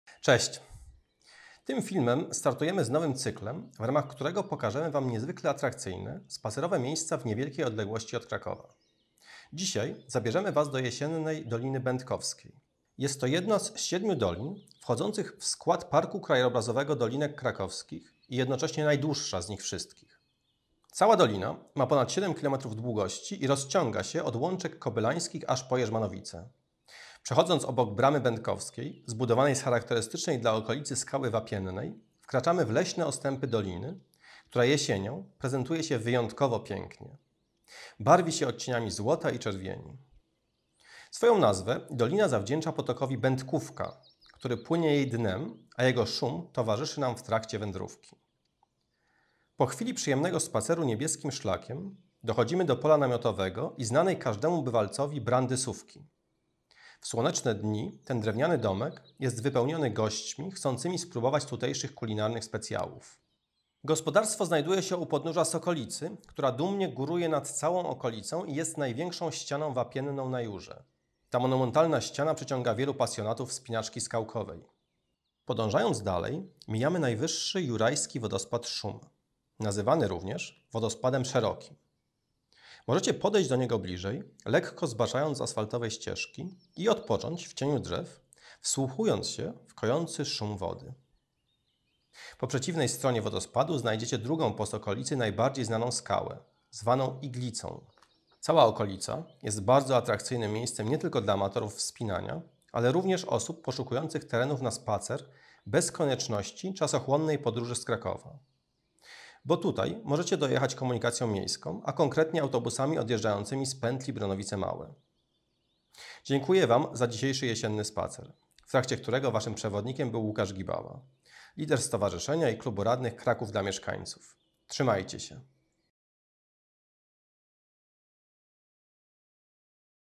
Dolina Będkowska
Tym filmem startujemy z nowym cyklem, w ramach którego pokażemy Wam niezwykle atrakcyjne, spacerowe miejsca w niewielkiej odległości od Krakowa. Dzisiaj zabierzemy Was do jesiennej Doliny Będkowskiej. Jest to jedna z siedmiu dolin wchodzących w skład Parku Krajobrazowego Dolinek Krakowskich i jednocześnie najdłuższa z nich wszystkich.